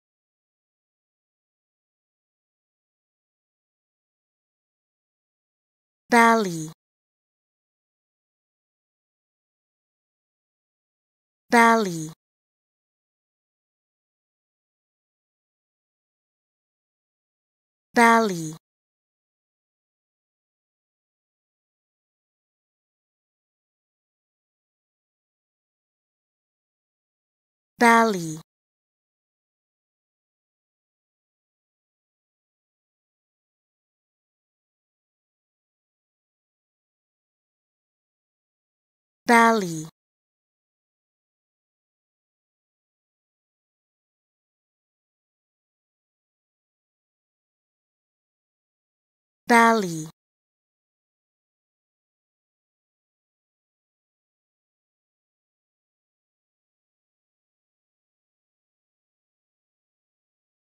How To Say Bally
Learn how to say Bally with EmmaSaying free pronunciation tutorials.